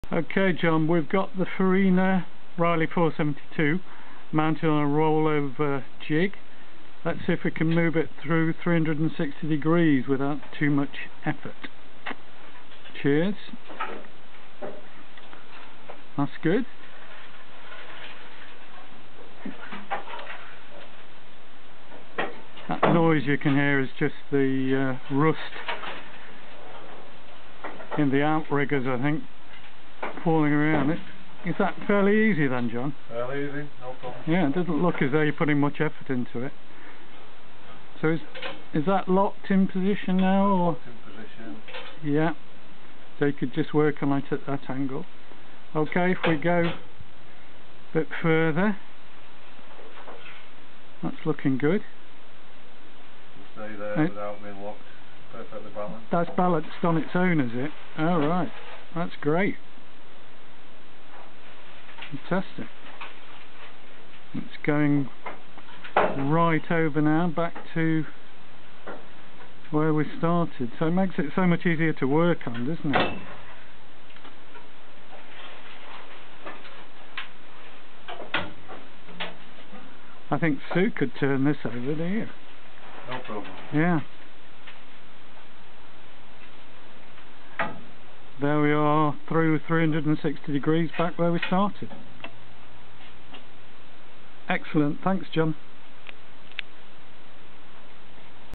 Riley 4/72 Farina body shell rotating through 360 degrees on a Rolloverjig
Video showing how easy it is to rotate a Farina body shell through 360 degrees on a Rolloverjig. The shell has been stripped of all mechanical and suspension parts plus doors, bonnet and boot lid.